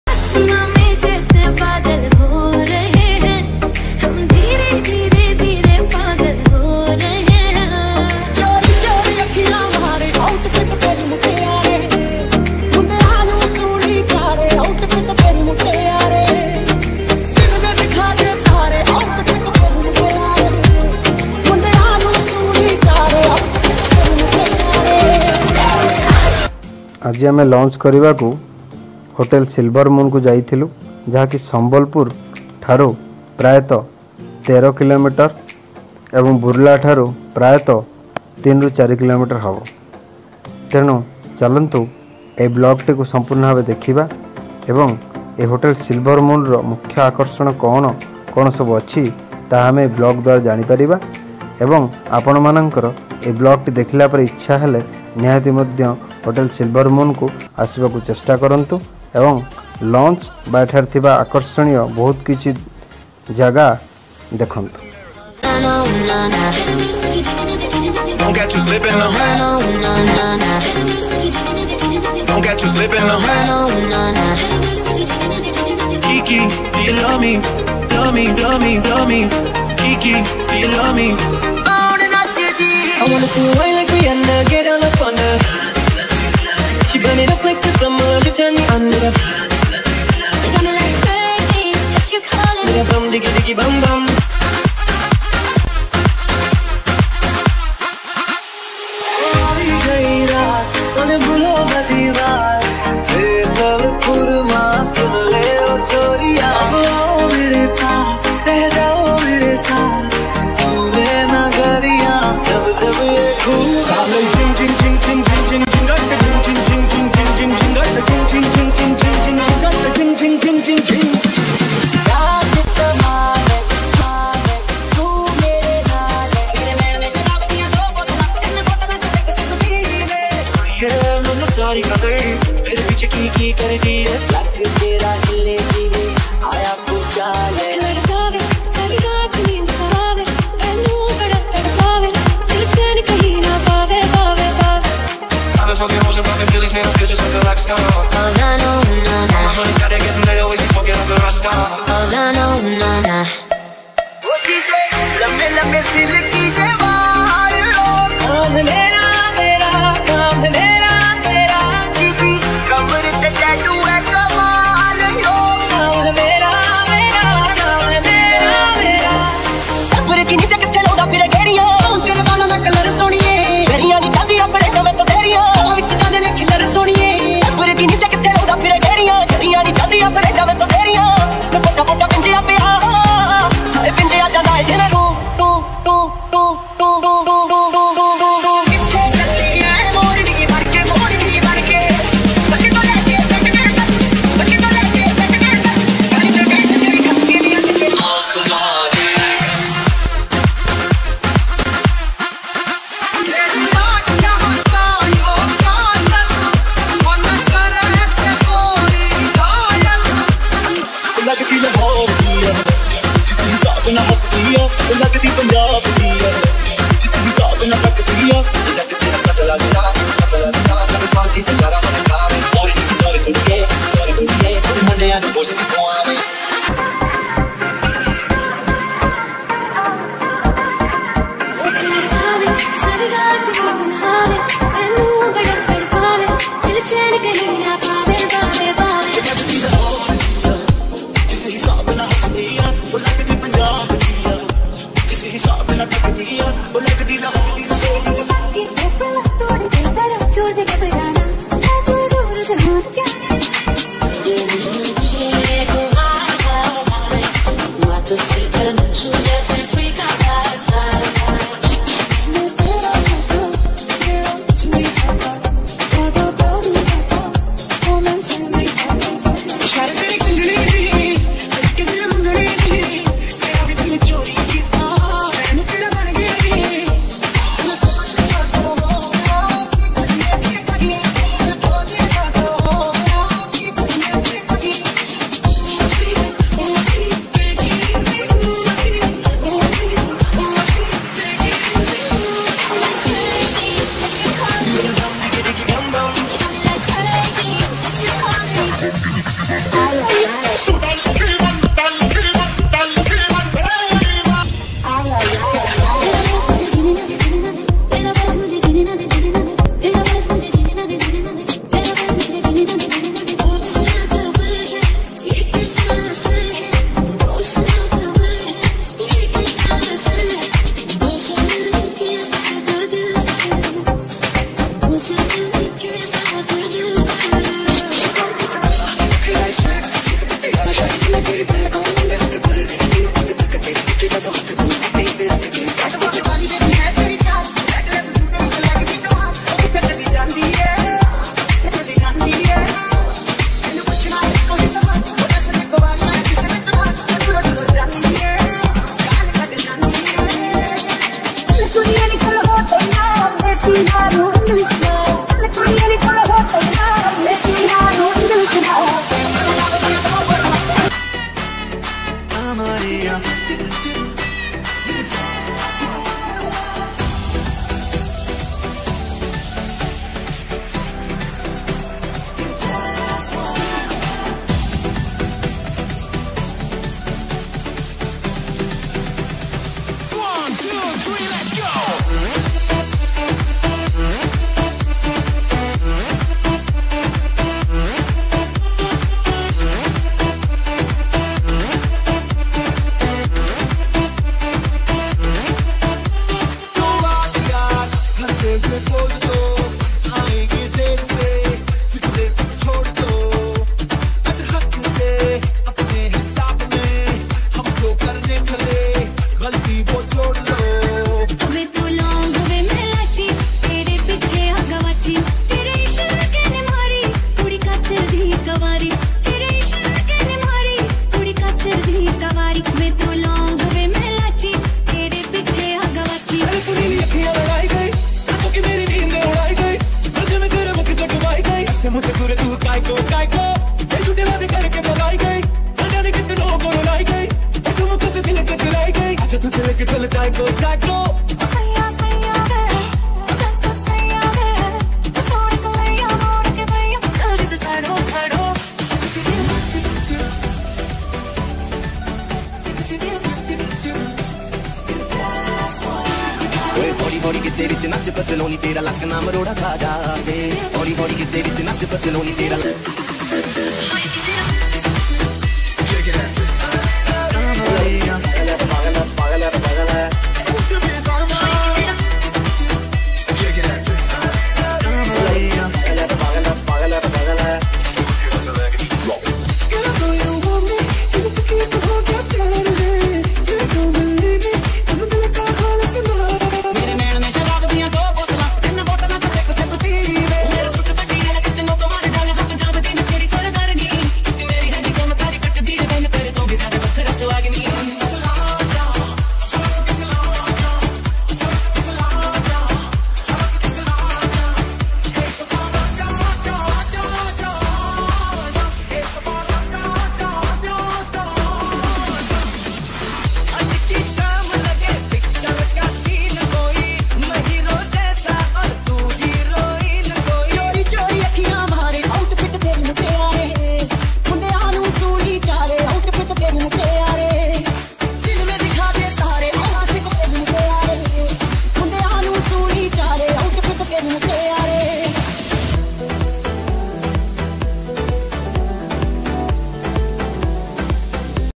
Swimming pool party||Sambalpur best hotel to stay||Best hotel in Sambalpur
This swimming pool name is 7 star swimming pool .its have inside hotelsilvermoon,sambalpur burla.
its around 14 km from sambalpur town towards burla